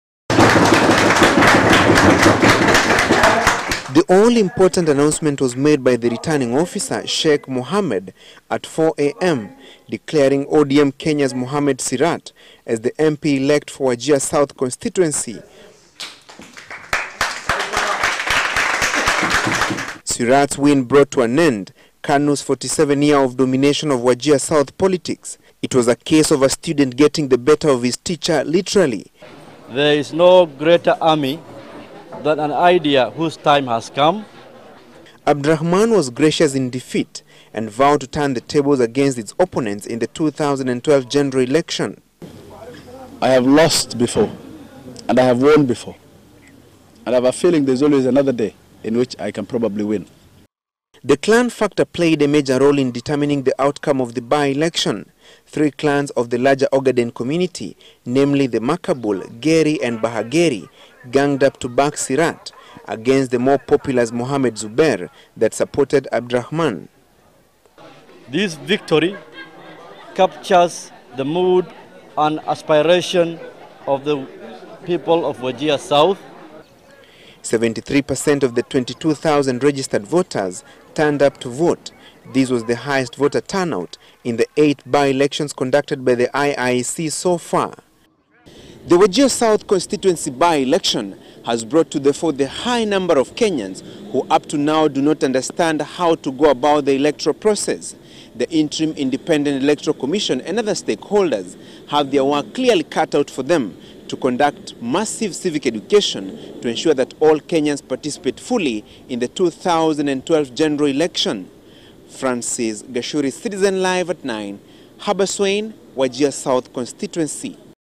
Wajir South MP Elect
Wednesday's victory of ODM Kenya's candidate Mahmud Sirat in Wajir South constituency , has broken kANU's 47-year old political control of the constituency. The KANU flag-bearer Abdirahman Ali Hassan acknowledged defeat, claiming that his rivals in 2007 ganged up against him, to ensure that his reign comes to a halt. Francis Gachuri reports on the salient factors that influenced the by election outcome.